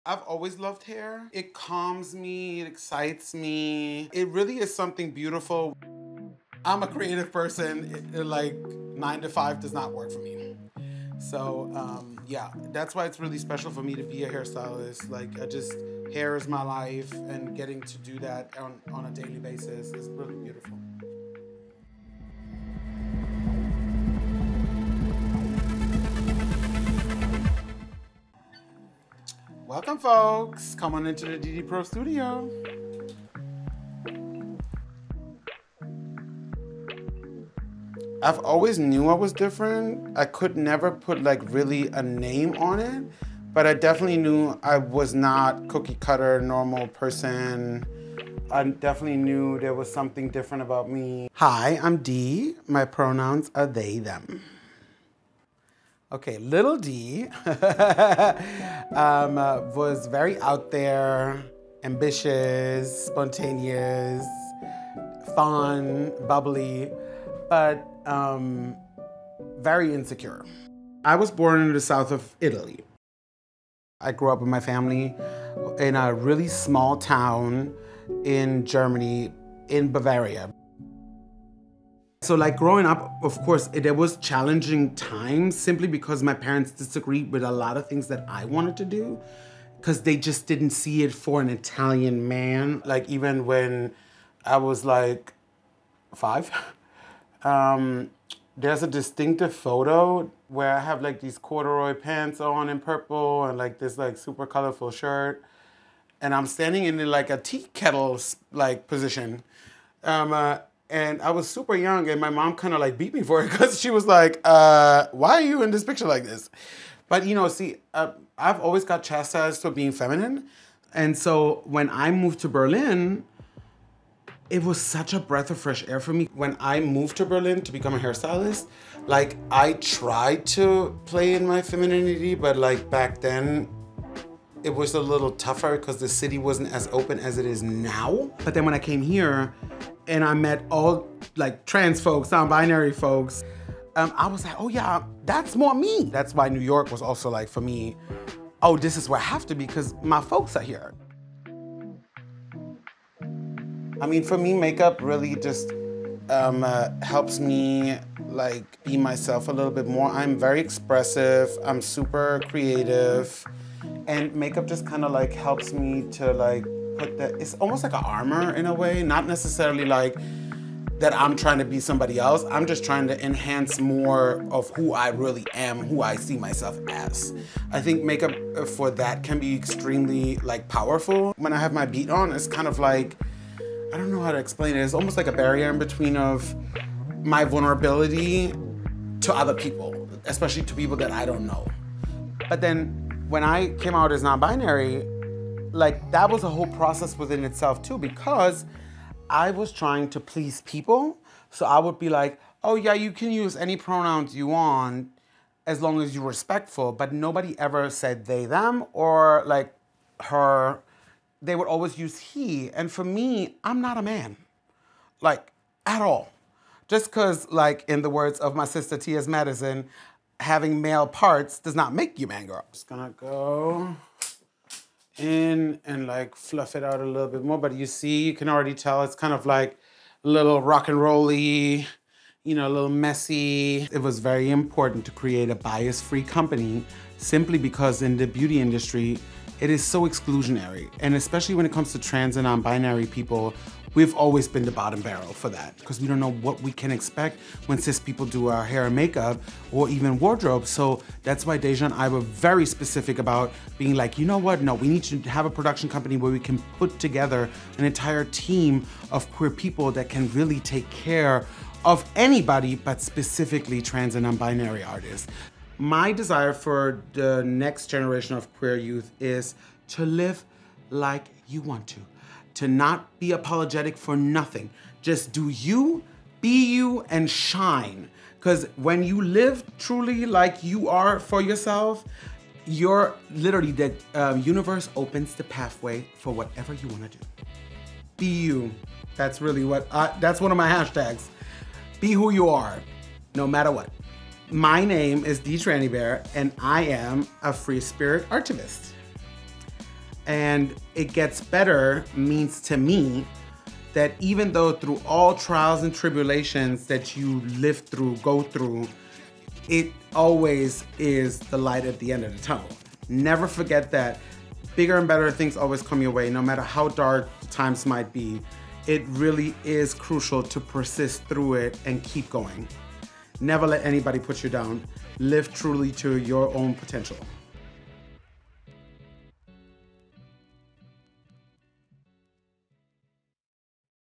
Why I Stopped Trying to Please People | It Gets Better: Dee Trannybear
"Artivist" Dee Trannybear-Marino found themself through hair and makeup, but saw that the beauty industry didn't really cater to trans and nonbinary clients...so they co-founded a company that does just that! 💄✂️

🏳️‍🌈 🏳️‍⚧️ The It Gets Better Project exists to uplift, empower, and connect LGBTQ+ youth around the globe.